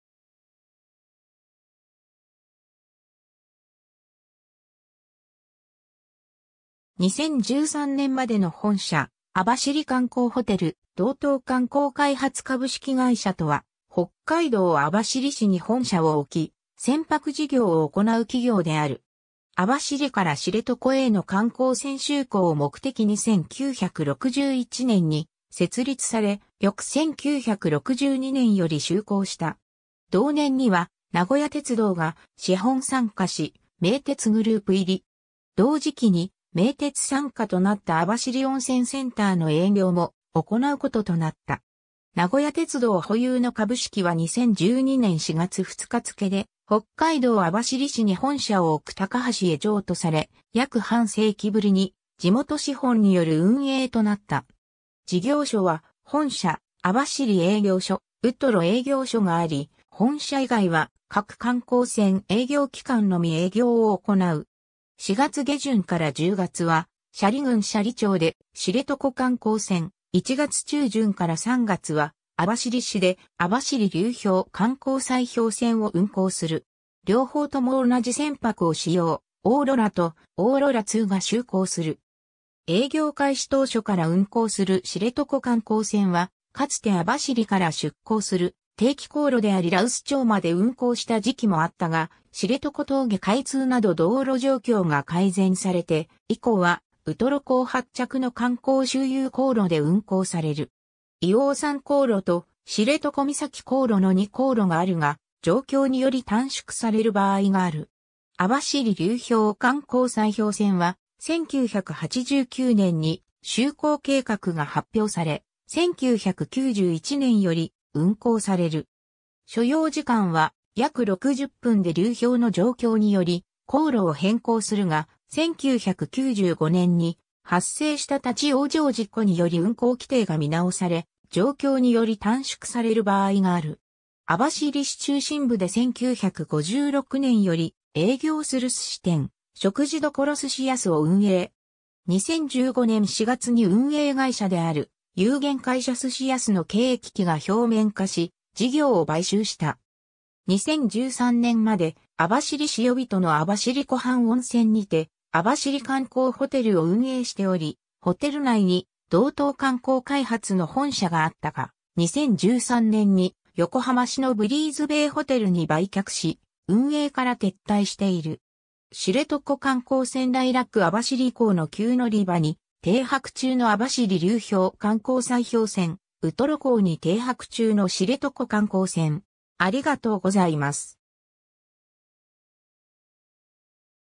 道東観光開発
2013年までの本社・網走観光ホテル 道東観光開発株式会社（どうとうかんこうかいはつ）とは、北海道網走市に本社を置き、船舶事業を行う企業である。
 網走から知床への観光船就航を目的に1961年（昭和36年）に設立され、翌1962年（昭和37年）より就航した。
同年には名古屋鉄道が資本参加し名鉄グループ入り。
同時期に名鉄傘下となった網走温泉センター（網走観光ホテルの前身）の営業も行うこととなった。
 名古屋鉄道保有の株式は2012年（平成24年）4月2日付で北海道網走市に本社を置くタカハシへ譲渡され、約半世紀ぶりに地元資本による運営となった。
 事業所は本社、網走営業所、ウトロ営業所があり、本社以外は各観光船営業期間のみ営業を行う。
 4月下旬 - 10月は斜里郡斜里町で知床観光船、1月中旬 - 3月は網走市で網走流氷観光砕氷船を運航する。
両方とも同じ船舶を使用、「おーろら」と「おーろらII」が就航する。
 営業開始当初から運航する知床観光船は、かつて網走から出航する定期航路であり羅臼町まで運航した時期もあったが、知床峠開通など道路状況が改善されて以降はウトロ港発着の観光周遊航路で運航される。
硫黄山航路（所要時間約1時間30分）と知床岬航路（所要時間約3時間45分）の2航路があるが、状況により短縮される場合がある。
 網走流氷観光砕氷船は、1989年（平成元年）に就航計画が発表され、1991年（平成3年）より運航される。
所要時間は約60分で流氷の状況により航路を変更するが、1995年（平成7年）に発生した立ち往生事故により運航規定が見直され、状況により短縮される場合がある。
 網走市中心部で1956年（昭和31年）より営業する寿司店、食事処 寿し安を運営。
2015年（平成27年）4月に運営会社である有限会社寿し安の経営危機が表面化し、事業を買収した。
 2013年まで、網走市呼人の網走湖畔温泉にて網走観光ホテルを運営しており、ホテル内に道東観光開発の本社があったが、2013年に横浜市のブリーズベイホテルに売却し運営から撤退している。
 知床観光船 ライラック（1990年） 網走港の旧乗り場に停泊中の網走流氷観光砕氷船（2007年） ウトロ港に停泊中の知床観光船（2011年）